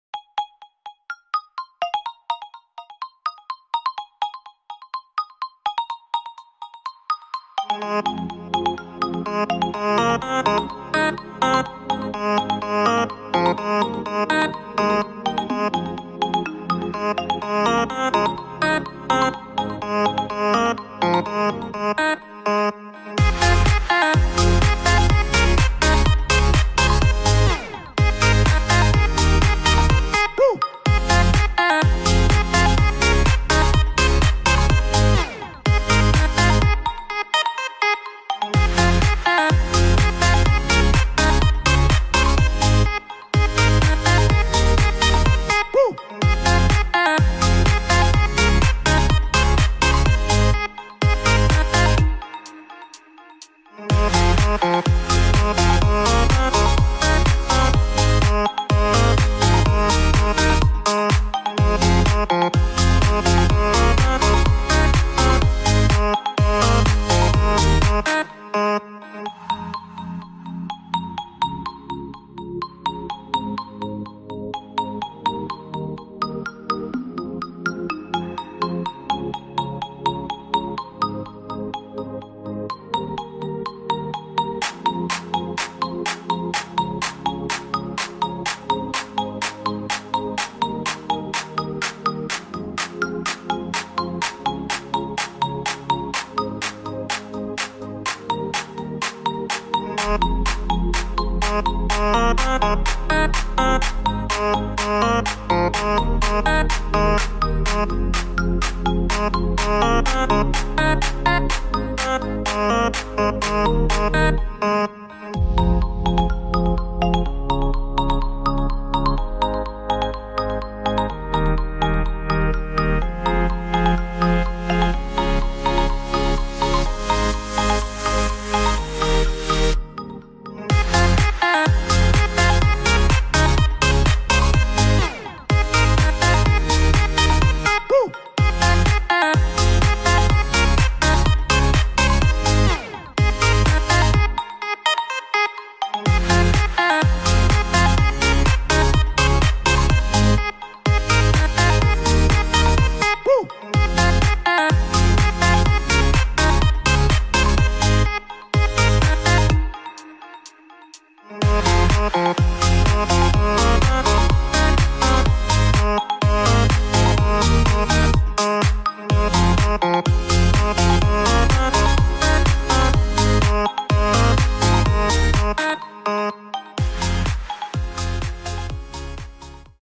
Pixelated Disney Character Quiz Answers ~ Quiz Diva Disney Characters
This video shows you how to get 100% on the Pixelated Disney Character Quiz.